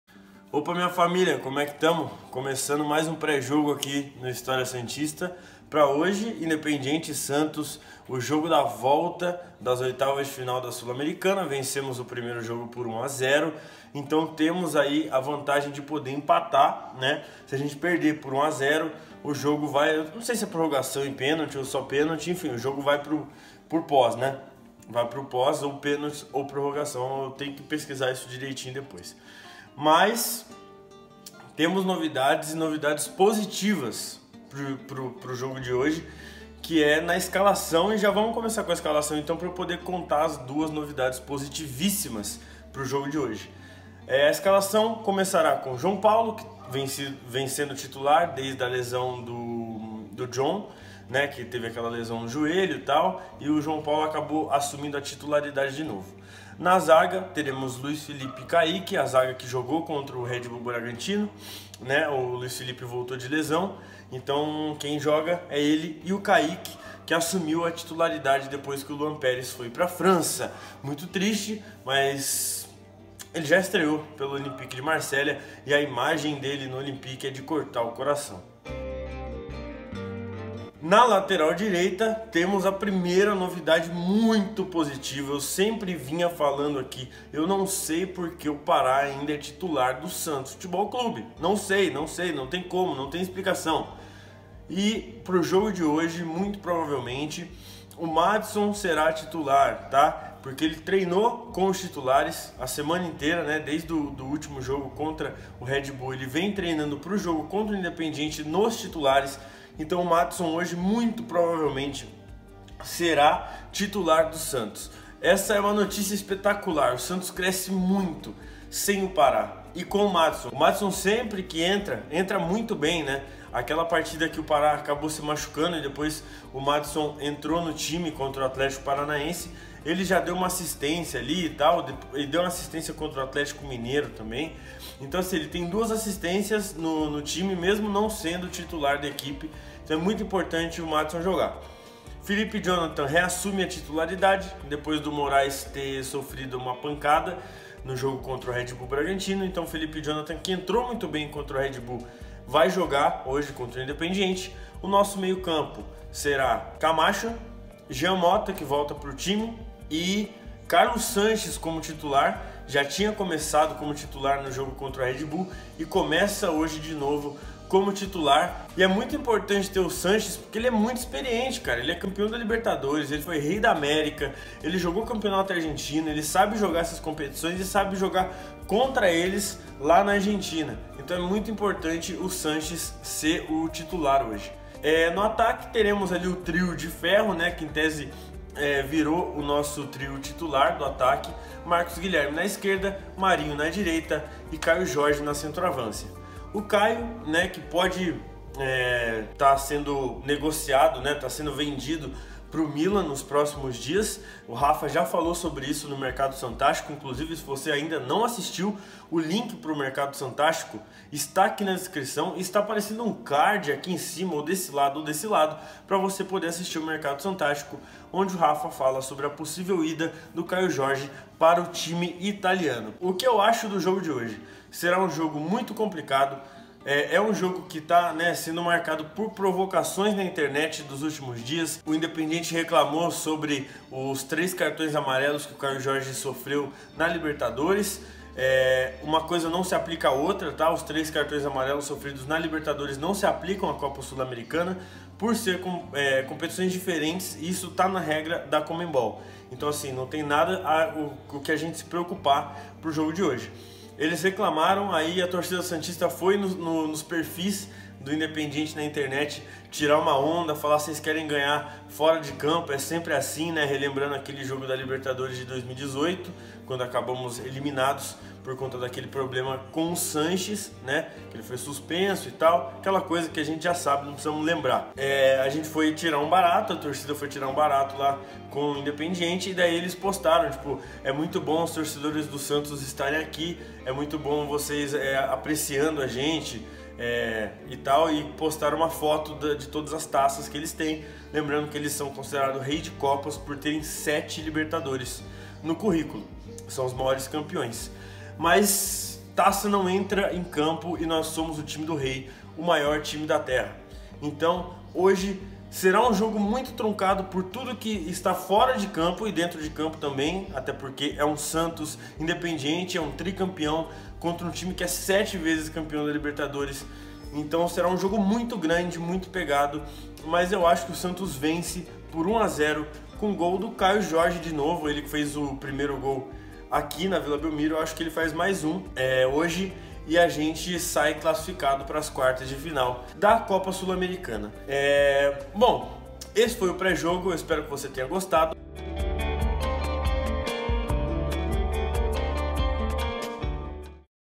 PRÉ-JOGO - INDEPENDIENTE X SANTOS
Hoje é decisão, hoje é Brasil x Argentina. Pra você ficar por dentro de tudo sobre a decisão de hoje do Peixão pela Sul-americana, assista ao Pré-Jogo com o Arpogon!